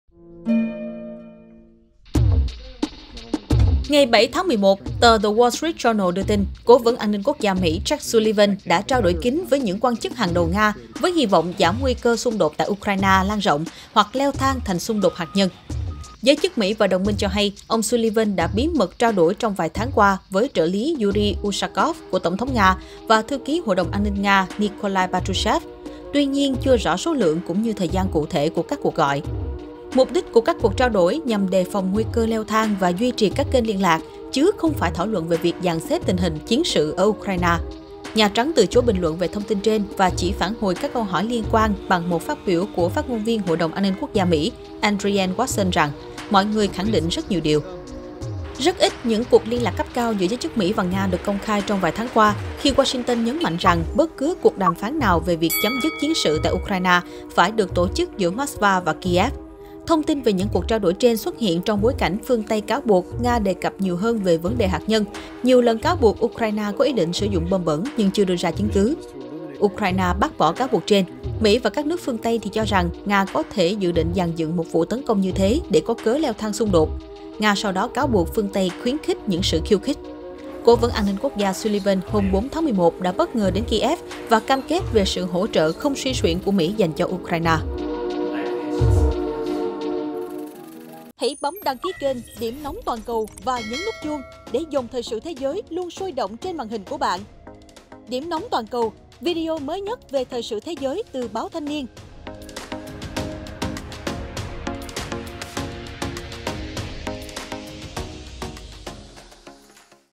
Nga Mỹ bí mật trao đổi về tình hình Ukraine?
Ngày 7.11, tờ The Wall Street Journal đưa tin Cố vấn An ninh quốc gia Mỹ Jake Sullivan đã trao đổi kín với những quan chức hàng đầu Nga liên quan đến tình hình xung đột ở Ukraine.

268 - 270 Nguyễn Đình Chiểu, Phường Võ Thị Sáu, quận 3, TP HCM